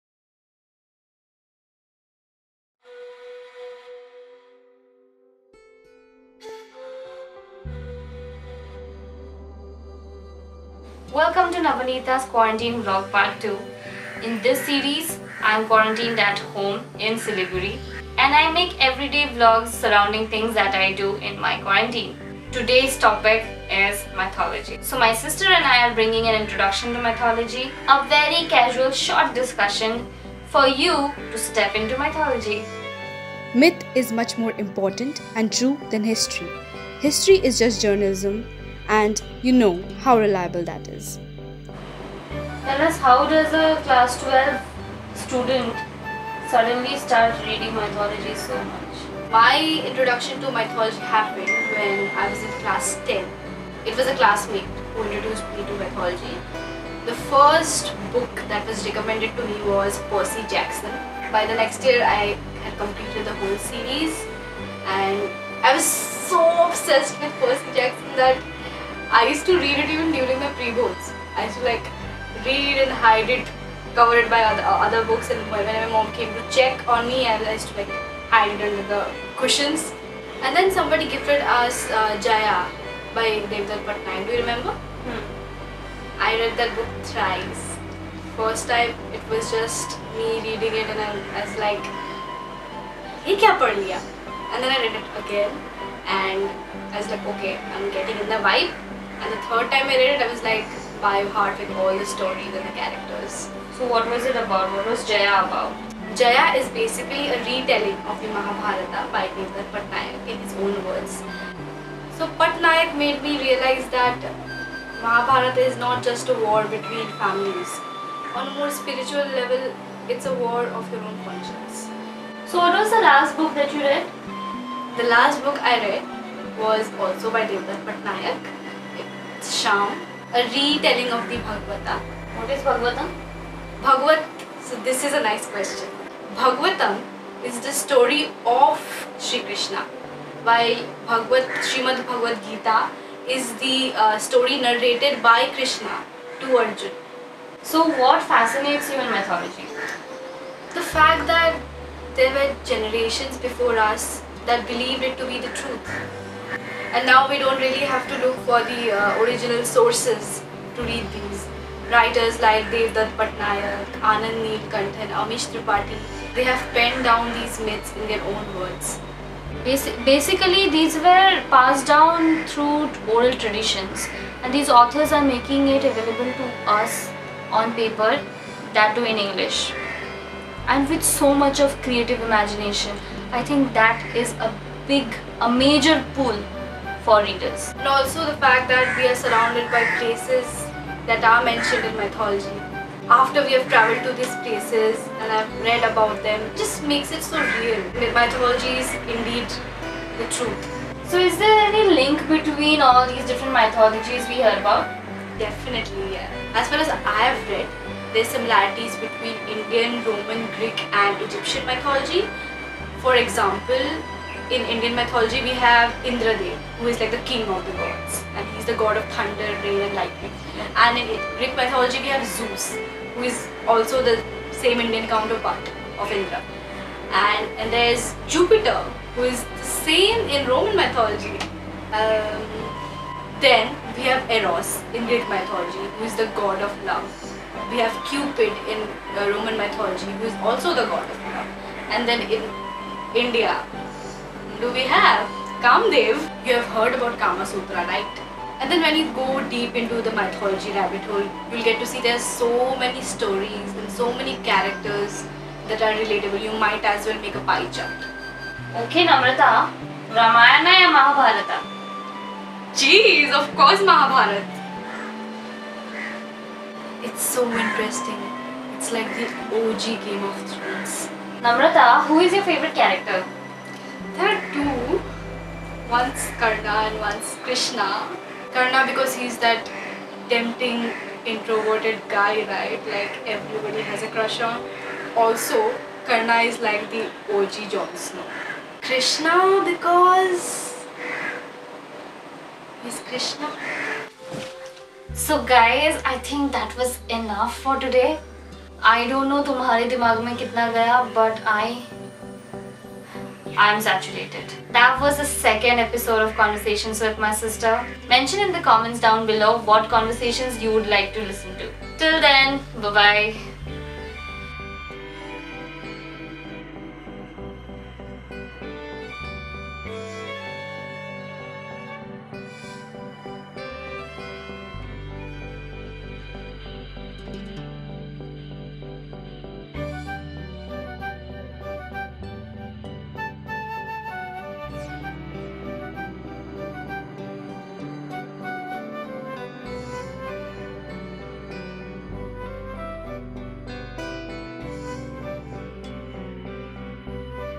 Conversations with my Sister | Mythology - an Introduction | HOME QUARANTINE VLOG | DAY 4
Mythology is an interesting field. A branch of history, it has stayed in our traditions for a long time. Today's mass media has made these myths so easily accessible to us that stories of Ram, Sita, Shiv are daily conversations in many households.
x
Music
Yellow Tunes
My Equipments
Motorola ONE
Gorilla Pod